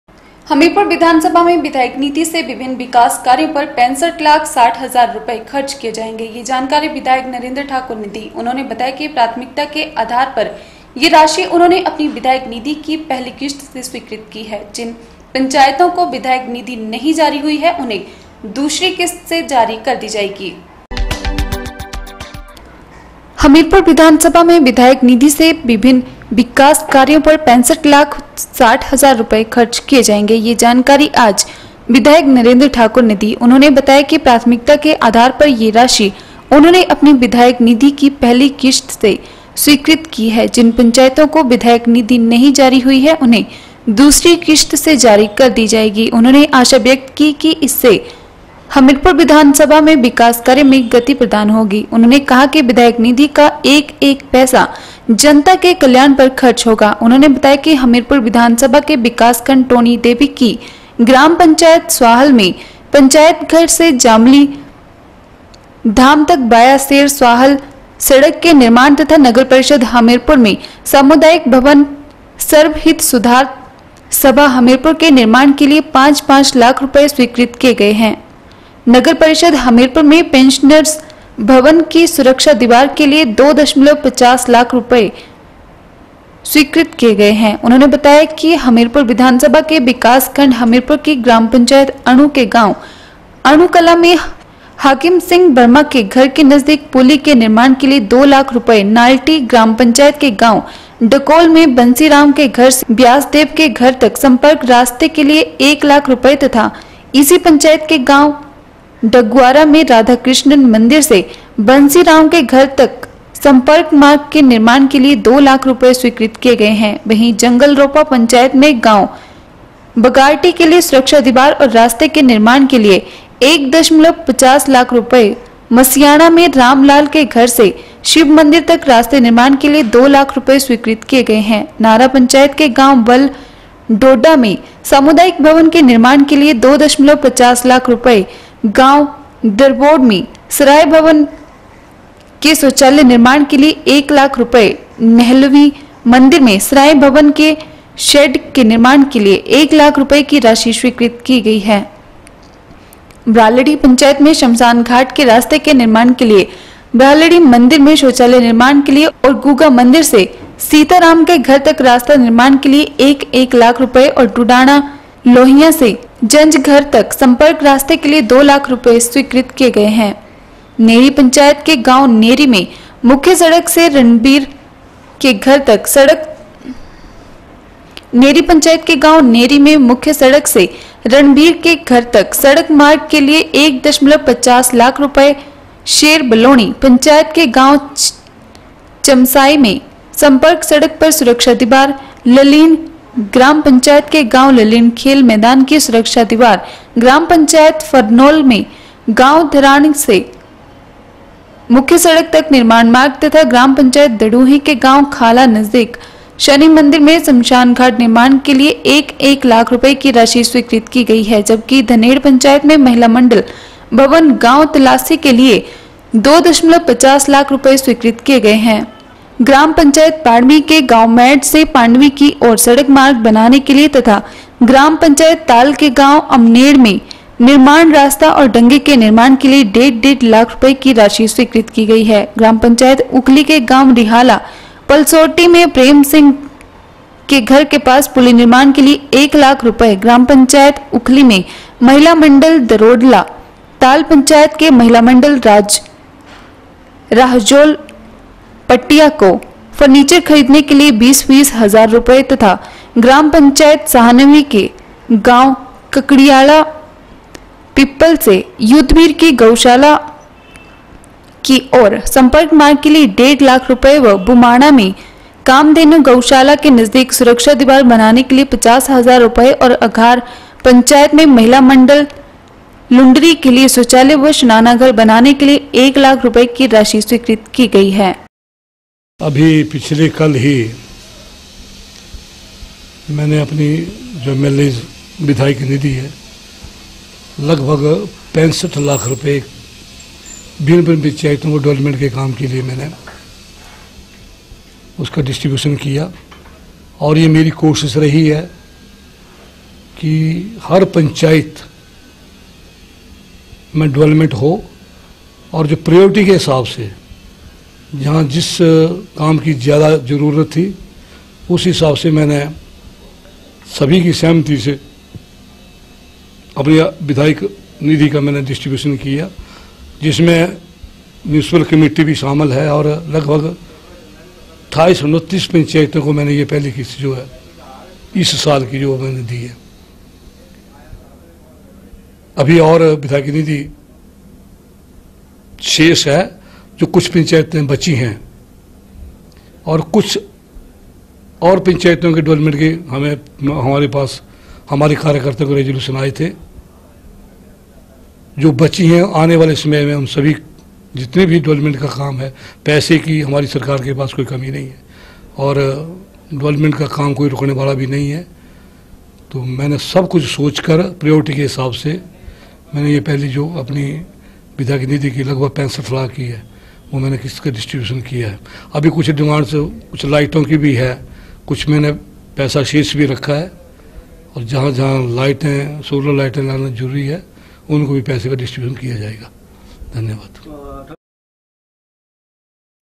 24 AUG N 5 65 lakh 60 thousand will be spent on various development works from the MLA fund.
हमीरपुर विधानसभा में विधायक निधि से विभिन्न विकास कार्यों पर 65 लाख 60 हजार  रुपये खर्च किए जायेंगे। यह जानकारी विधायक नरेन्द्र ठाकुर ने दी। उन्होंने बताया कि प्राथमिकता के आधार पर यह राशि उन्होंने अपनी विधायक निधि की पहली किश्त से स्वीकृत की है। जिन पंचायतों को विधायक निधि नहीं जारी हुई है उन्हें दूसरी किश्त से जारी कर दी जाएगी